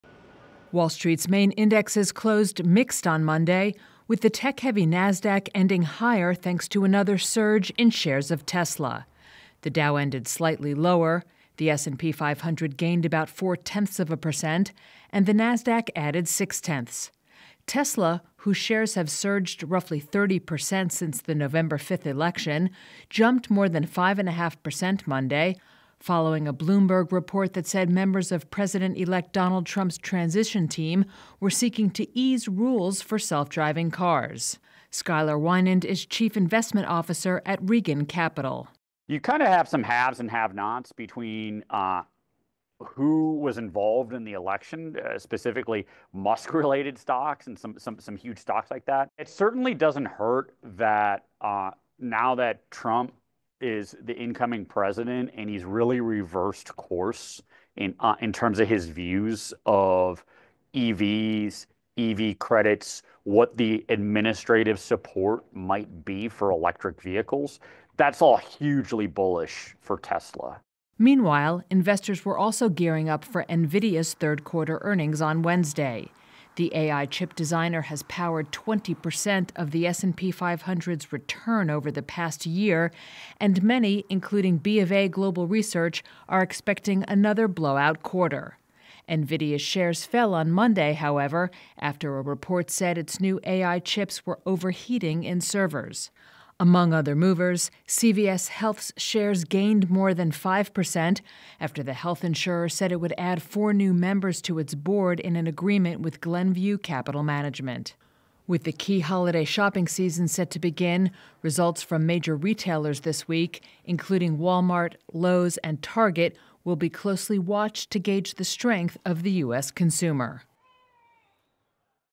Nasdaq, S&P 500 close higher as investors await Nvidia results | REUTERS
The Nasdaq and S&P 500 closed higher, recovering some losses as investors anticipate quarterly earnings from AI leader Nvidia, and Tesla shares jumped on the prospect of favorable policy changes from the incoming Trump administration.